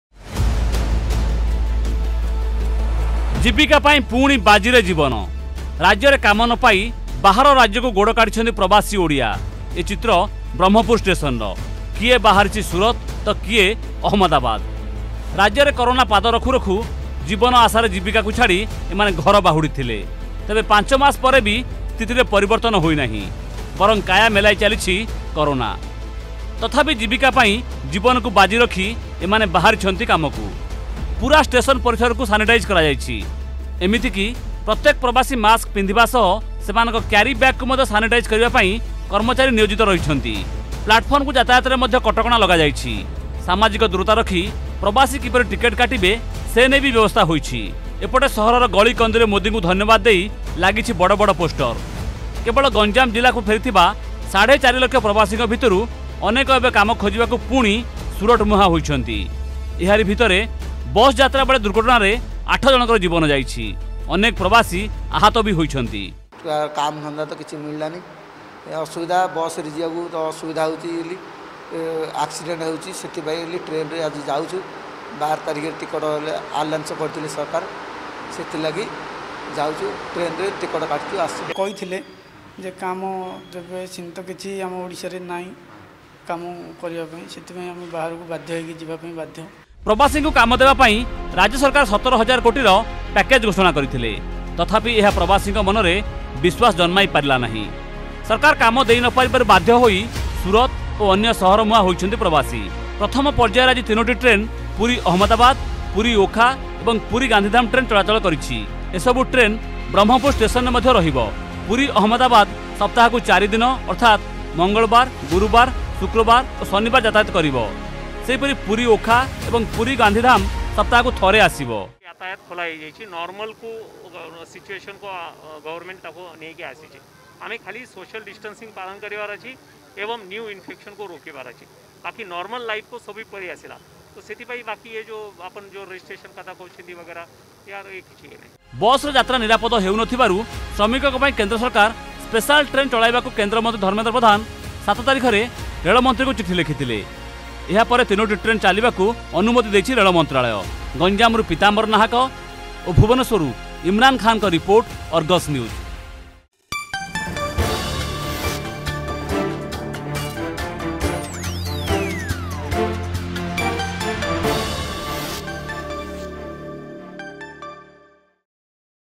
ପ୍ରବାସୀଙ୍କ ପାଇଁ ଆଜିଠାରୁ ପୁଣି ଗଡିଲା ଟ୍ରେନ..
ପ୍ରବାସୀଙ୍କ ପାଇଁ ଆଜିଠାରୁ ପୁଣି ଗଡିଲା ଟ୍ରେନ ।  ରାଜ୍ୟକୁ ଫେରିଥିବା ଲକ୍ଷ ଲକ୍ଷ ପ୍ରବାସୀ ପୁଣି କାମ ଅନ୍ୱେଷଣରେ ରାଜ୍ୟ ବାହାରକୁ ପାଦ କାଢିଛନ୍ତି ।  ବସରେ ଫେରିବା ନିରାପଦ ନ ହେବାରୁ କେନ୍ଦ୍ର ସରକାର ପ୍ରବାସୀଙ୍କ ପାଇଁ ଟ୍ରେନ ଚଲାଇବାକୁ ନିଷ୍ପତ୍ତି ନେବା ପରେ ଆଜି ପ୍ରଥମ ପର୍ୟ୍ୟାଯରେ ତିନୋଟି ଟ୍ରେନ ଚାଲିବା ଆରମ୍ଭ ହୋଇଛି ।